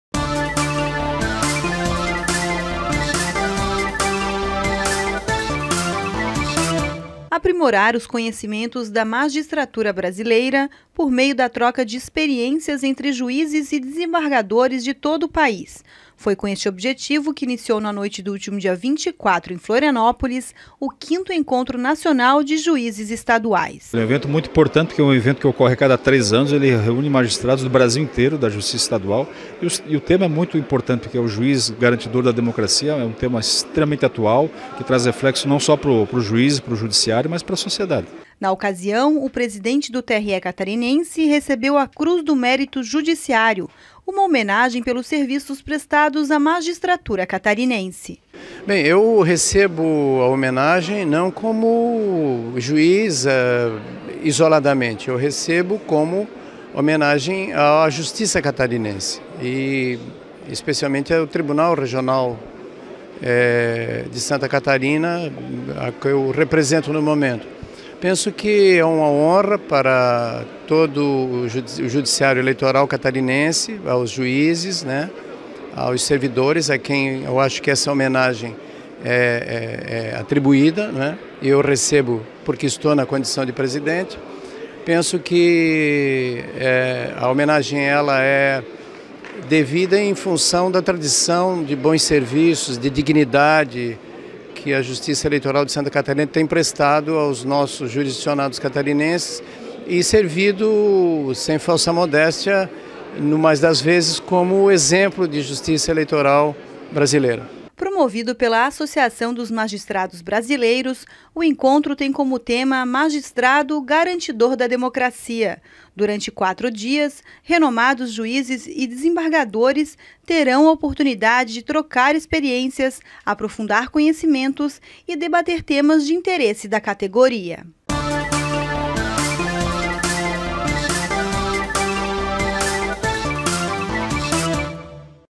"Mérito Judiciário" é concedido pela AMB e AMC ao presidente do TRE-SC
O presidente do TRE-SC (Tribunal Regional Eleitoral de Santa Catarina), desembargador Eládio Torret Rocha, foi homenageado durante a solenidade de abertura do V ENAJE (Encontro Nacional de Juízes Estaduais), na noite da última quinta-feira (24), no Costão do Santinho, em Florianópolis. O evento é uma promoção da Associação dos Magistrados Brasileiros (AMB), com o apoio da Associação dos Magistrados Catarinenses (AMC) e apresenta uma intensa programação que se estenderá até o próximo domingo (27).